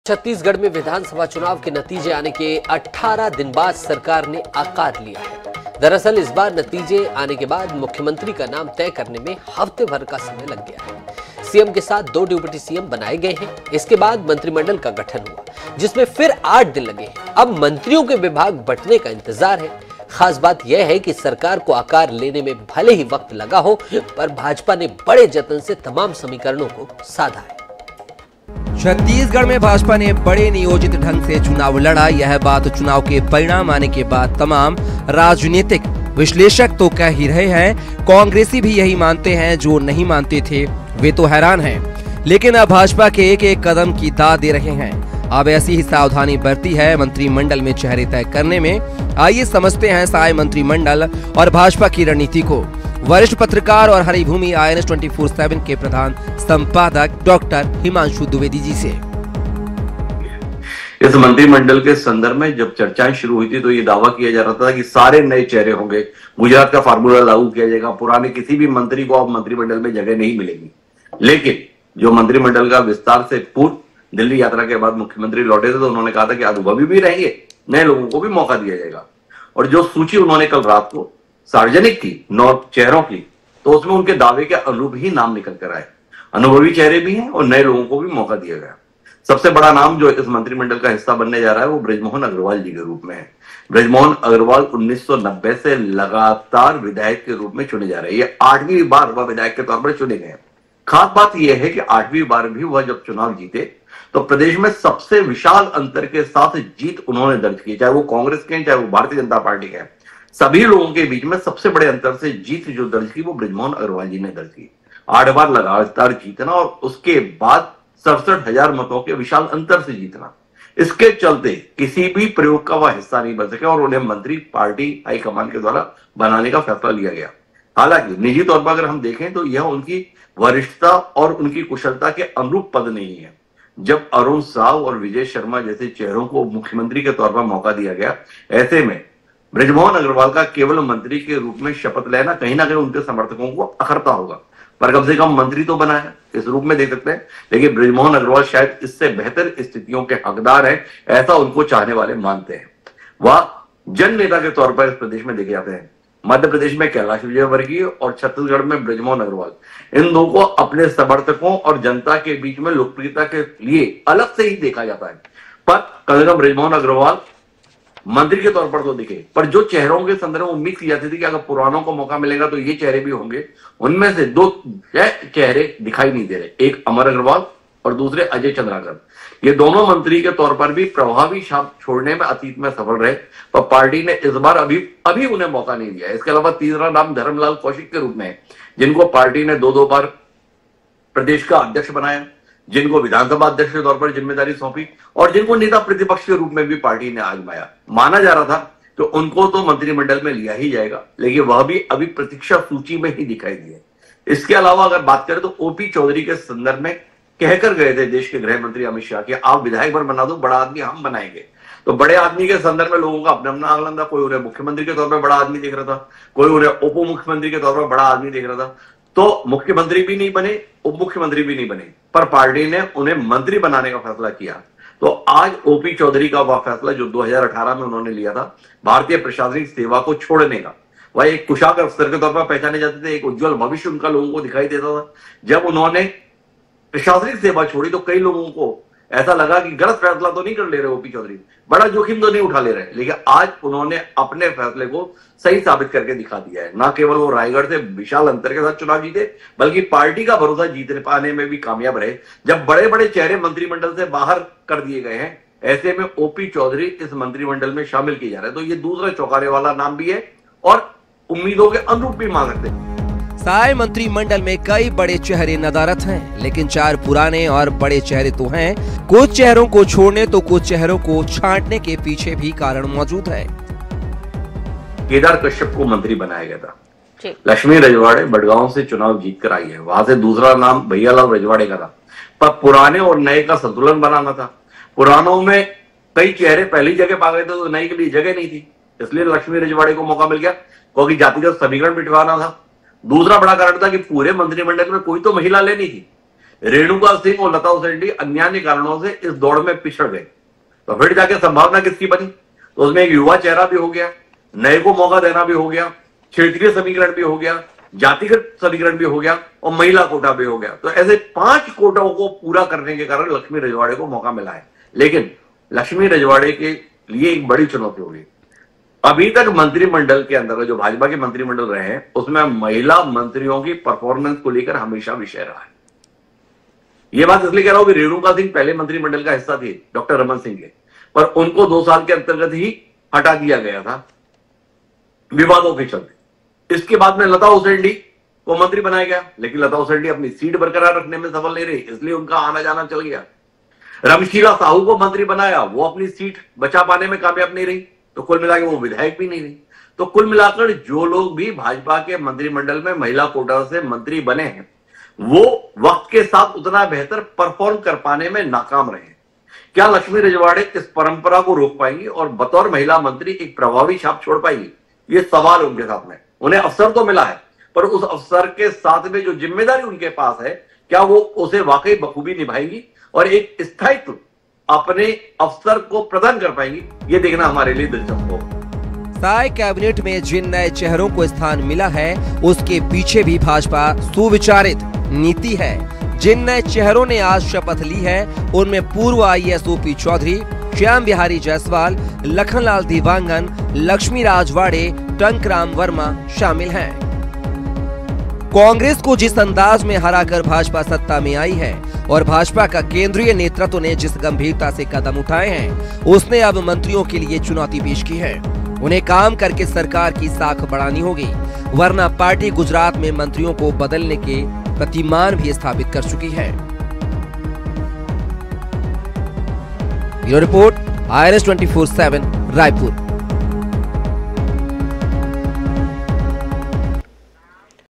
Sai Cabinet ने लिया आकार | केंद्रीय नेतृत्व की रहेगी नजर, नए और पुराने सदस्यों का संगम | CG BJP
INH, Sai Cabinet ने लिया आकार | केंद्रीय नेतृत्व की रहेगी नजर, नए और पुराने सदस्यों का संगम | CG BJP

Incoming Search Terms: